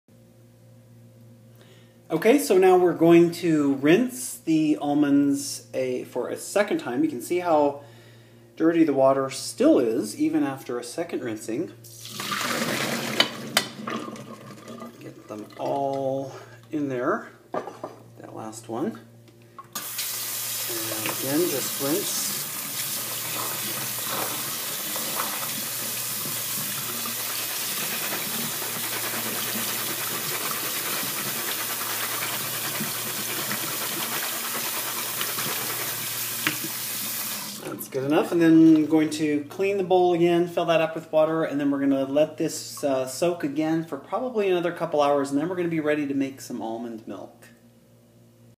Make Your Own Almond Milk Step Four: rinse again and continue soaking.
The key to soaking almonds is to end up with water that is clean and clear after having soaked for at least 12 hours. I end up rinsing 3 or 4 times.